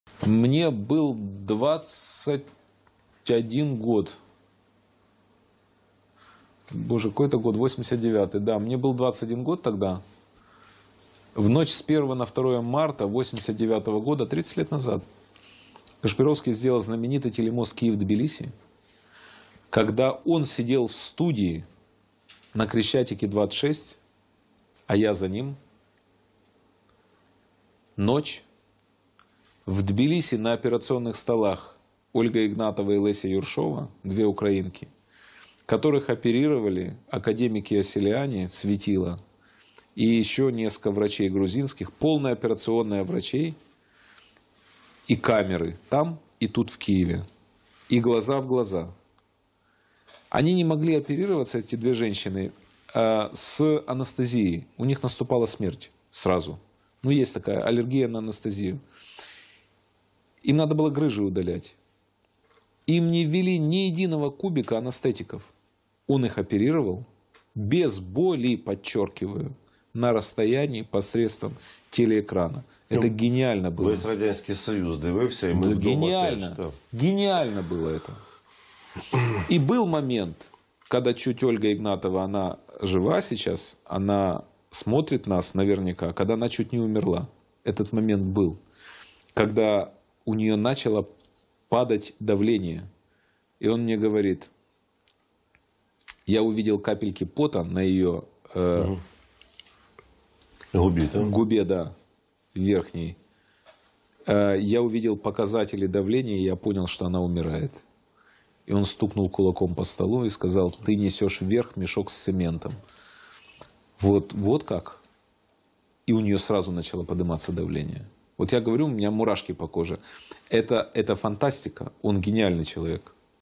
Гордон о телемосте "Киев-Тбилиси", где Кашпировский дистанционно обезболил пациенток на операции
Фрагмент интервью Дмитрия Гордона главному редактору телеканала "112-Украина" Павлу Кужееву